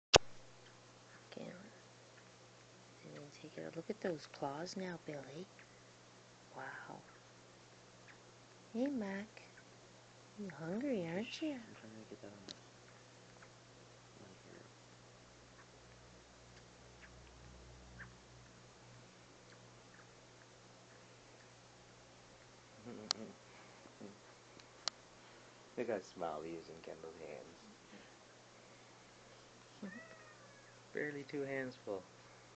Rescue kitten feeding with his best friend
The umbilical cord came off today and the eyes are still shut so the age is adjusted to approx 12 days old, younger then I had previously thought.